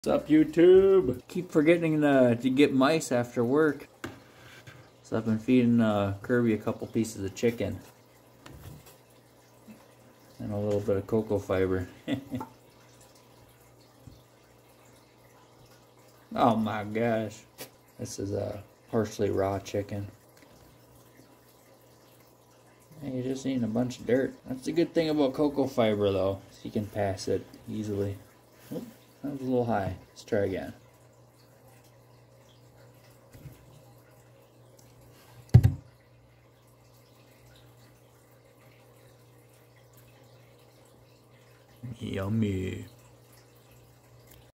Kerby and I both ate chicken tonight
Feeding my 15 year old Giant African Bullfrog, Kerby, some thawed chicken breast. I don't give him chicken very often, but when I can't make it to the pet store for mice or other goodies this is what he gets! Peace and love